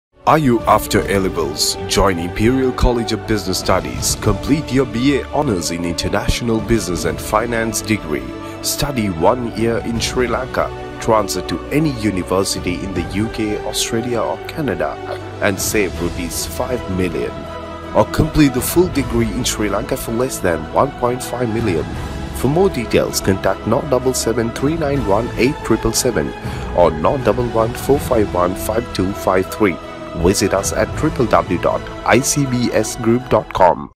Imperial College of Business Studies
Imperial College of Business Studies offers the only degree in the world to receive accreditation from 10 professional bodies including CIMA and ACCA